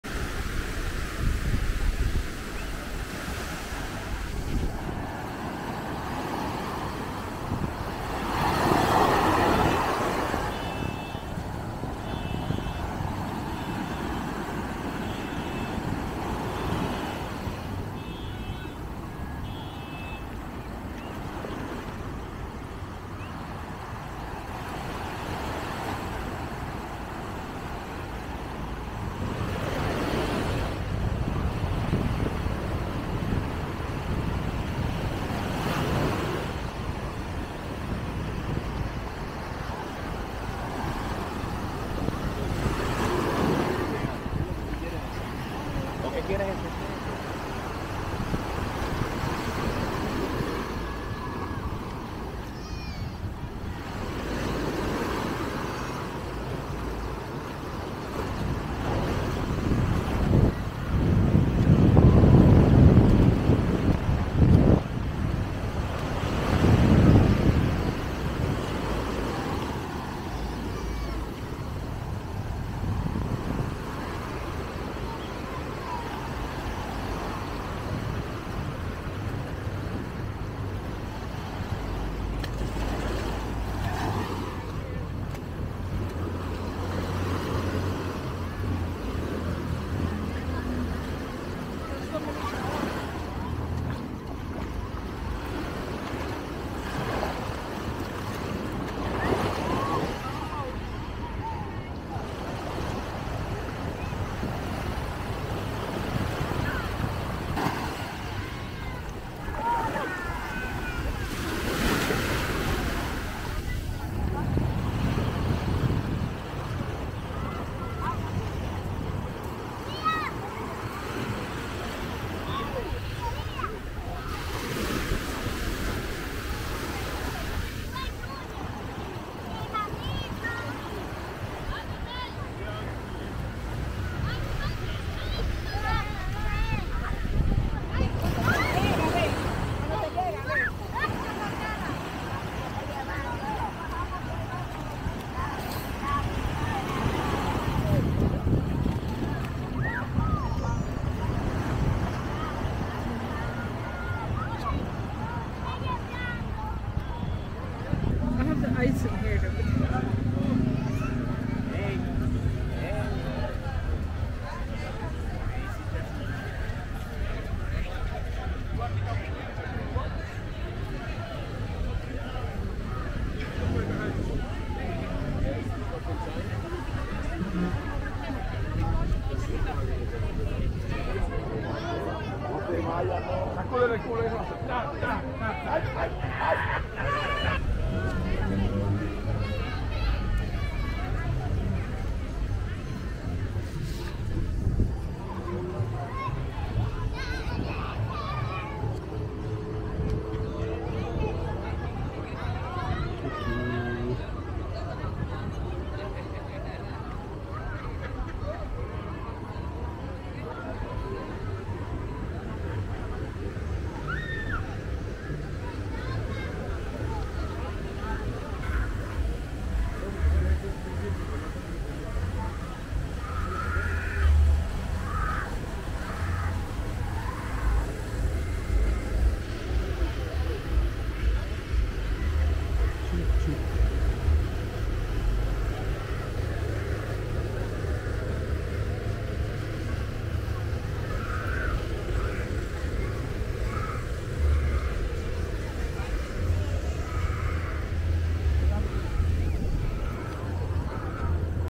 Puerto Plata Main Beach Tour Walking the beach High Quality Video
Puerto Plata Main Beach Tour Walking the beach High Quality Video Winter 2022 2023 Jan 2023. Exploring the beach, walking on the sand on the main Puerto plata beach